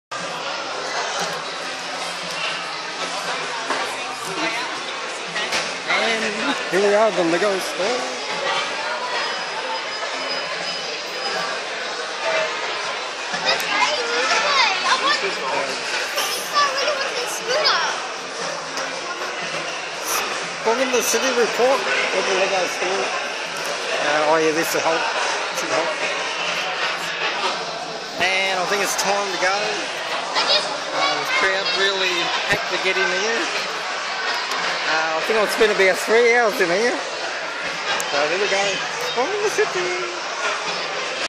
adam spong blog presents real lego store dreamworld review
adam spong  blogs once the people left the  store and  i could do  a proper  video,